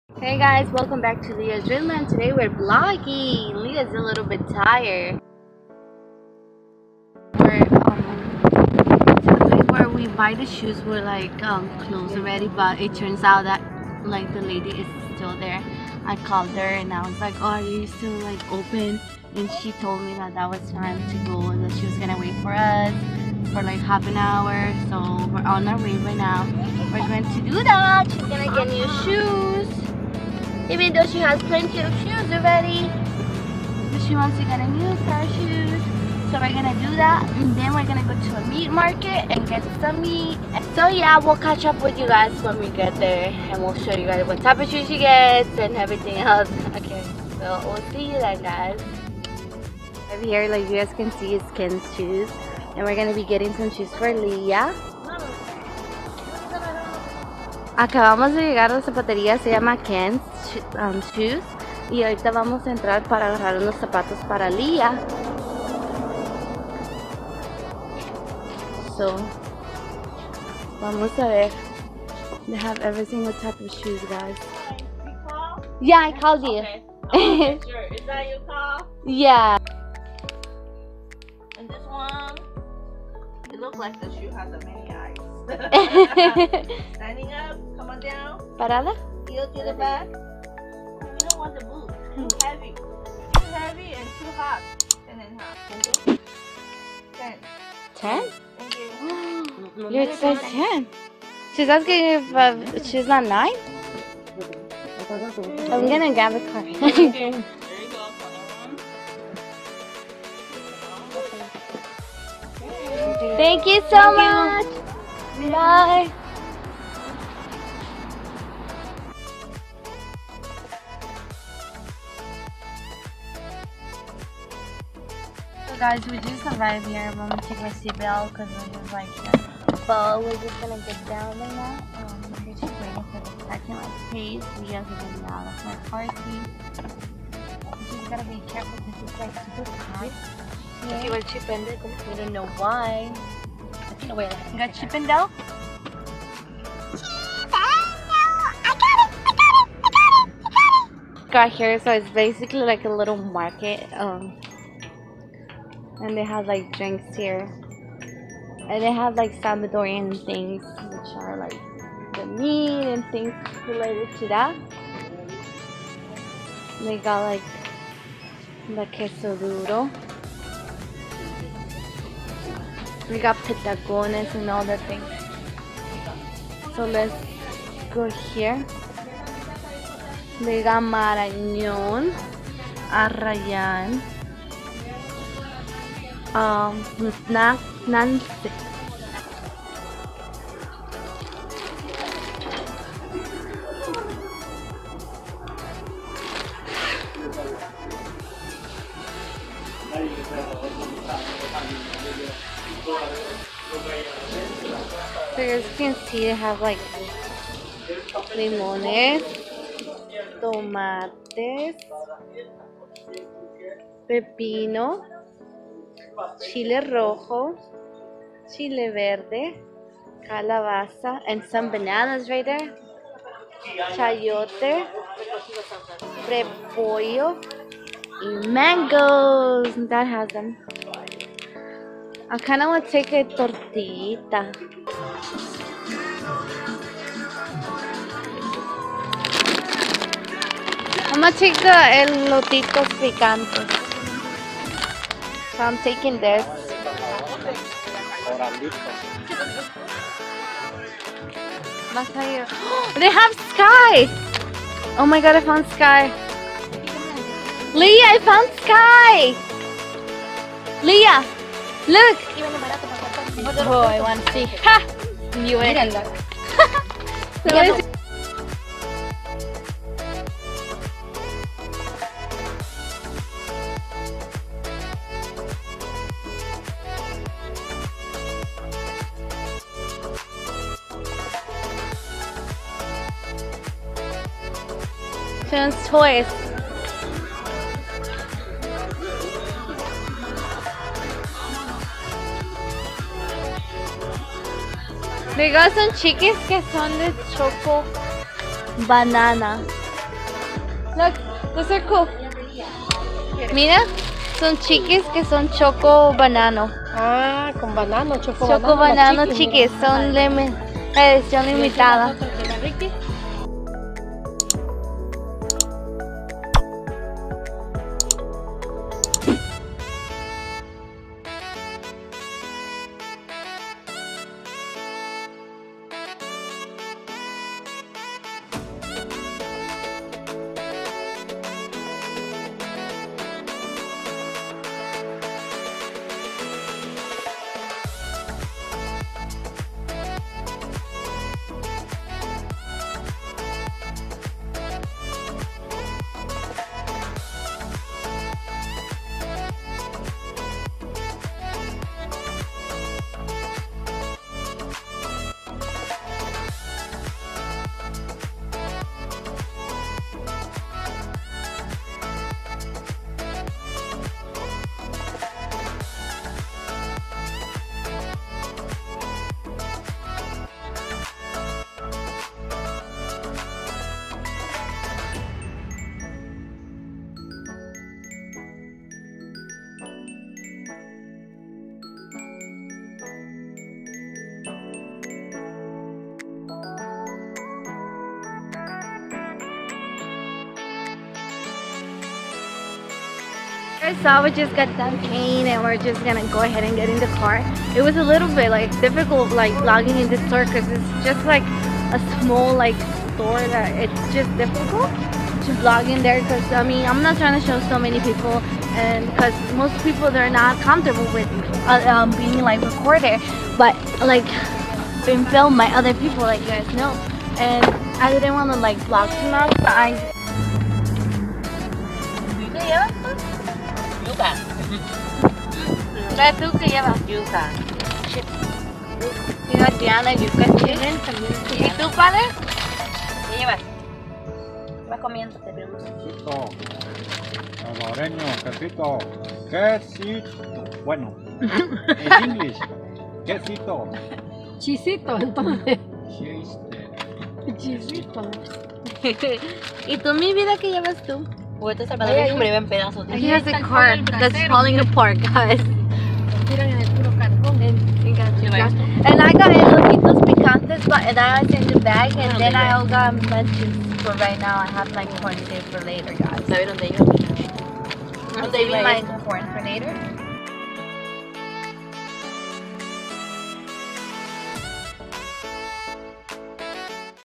Ken's SHOES & El Turco Meat Market
Hey Guys welcome back in this vlog we went to shop at Ken's SHOES in Pasadena and Visited  a Salvadorian Food Market called "El Turco Meat Market'".Lea had so much fun and we bought so many delicious goodies.As always don't forget to Subscribe if you are not already and till next time .Hugs and Blessing from your friends here at Lea's Dream Land.